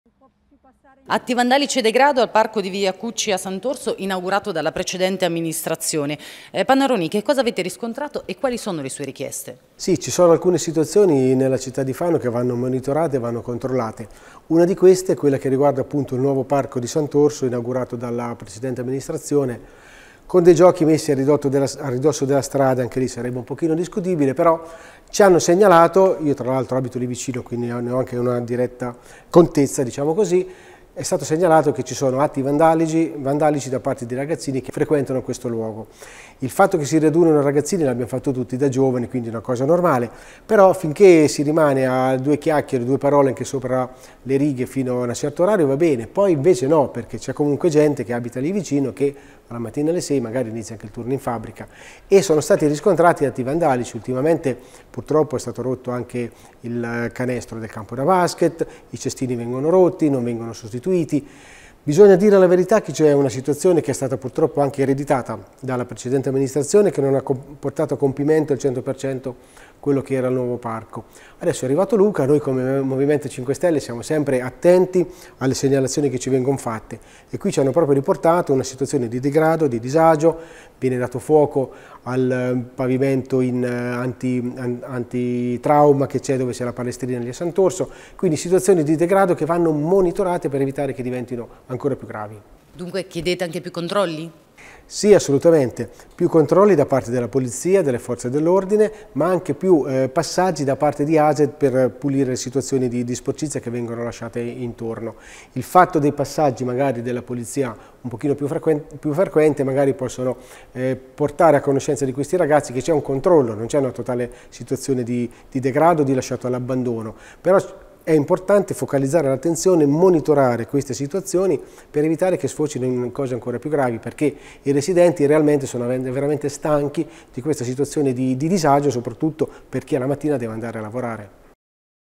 Panaroni: "Degrado e vandalismo al nuovo parco di Sant'Orso a Fano"
Atti vandalici, cestini insufficienti e degrado: il consigliere dei 5 Stelle, Francesco Panaroni, presenta un'interrogazione sullo stato di abbandono del nuovo parco di Sant'Orso a Fano.